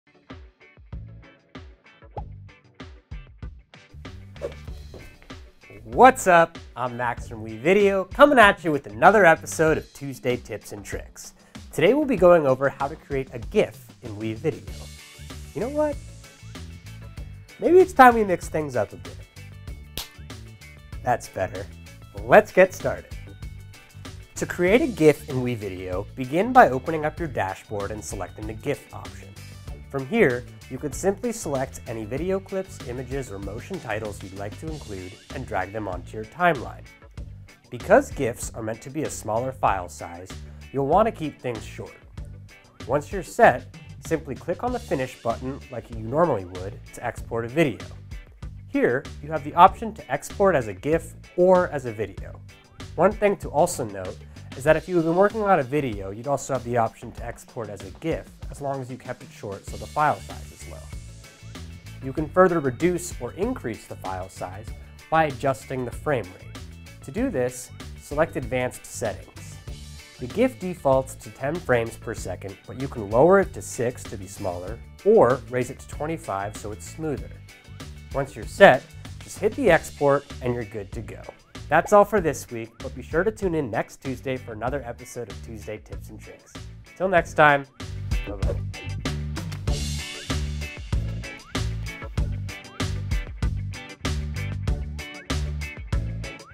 How to create GIFs in WeVideo
Learn how to quickly and easily create GIFs in WeVideo. From your dashboard you can now launch the WeVideo GIF creator or you can export short video clips as GIFs.